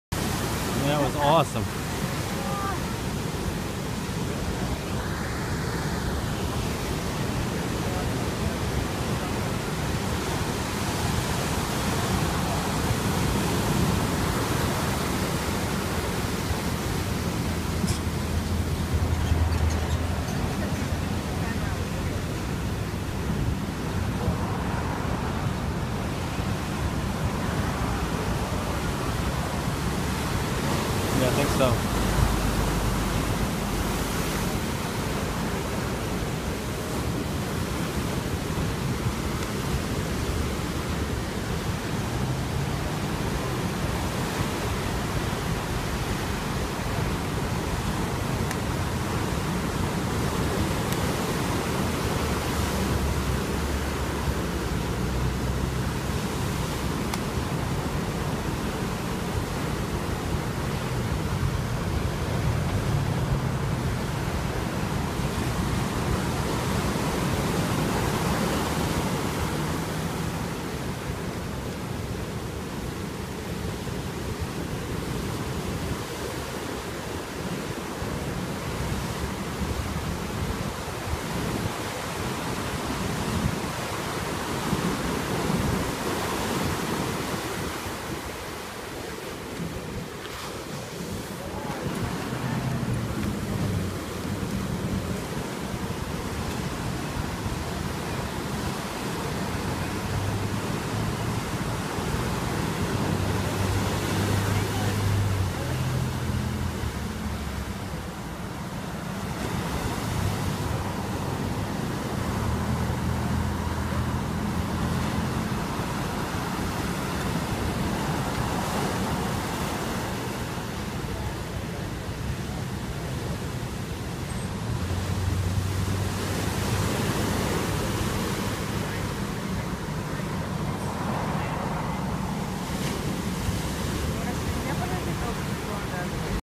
Watching people purposely get pounded by waves at Waimea Beach, north shore
We had fun watching the people put themselves in the path of the waves at Waimea Beach park on the North Shore.